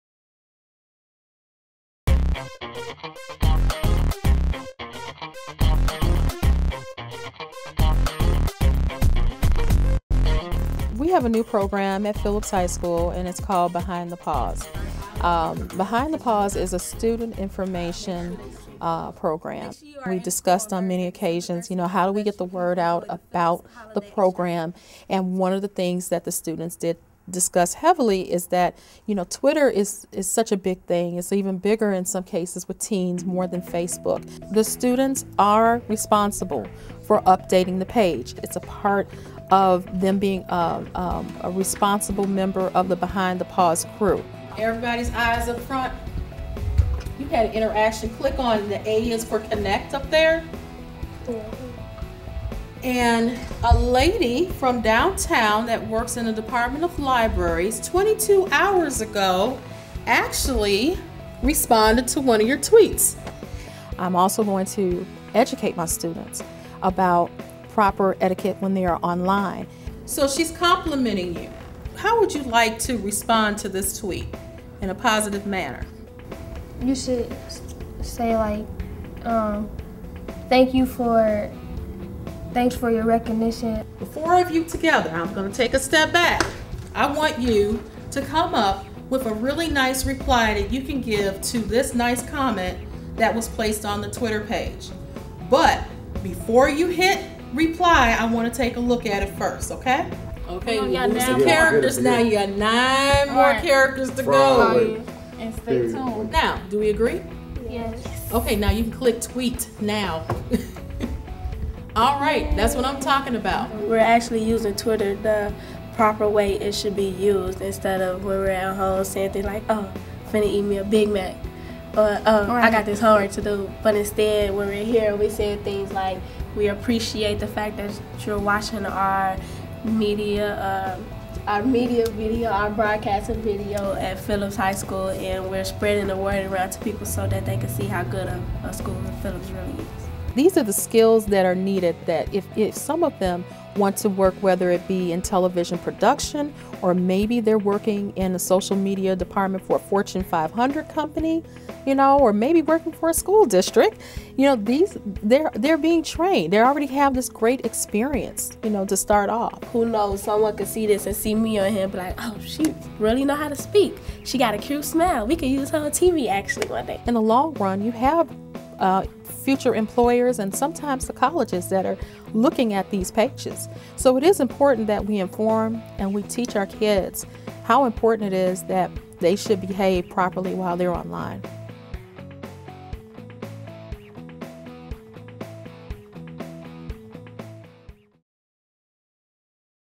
Phillips Academy High School - Social Media Trailblazers ("Teaching Social Media Etiquette")
Staff and students from Chicago's Phillips Academy High School show how they teach & learn social media etiquette.